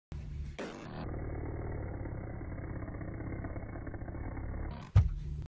Sucker Punched
Got sucker punched by my 5 year old